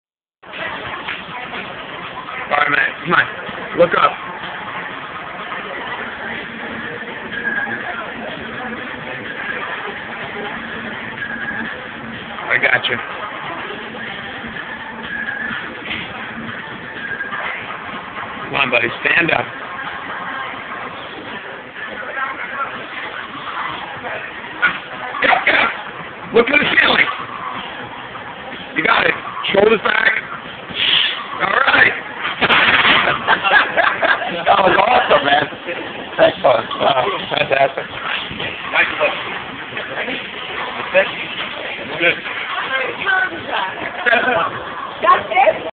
Matt Sohmer 700LBS deadlift w/ straps @ 17 yrs old
MATT SOHMER Sets unofficial deadlift record at the age 17 by deadlifting 700lbs in merrick under the supervision of Richard Seibert (Olympic trainer and Chiroprator) *
with straps ***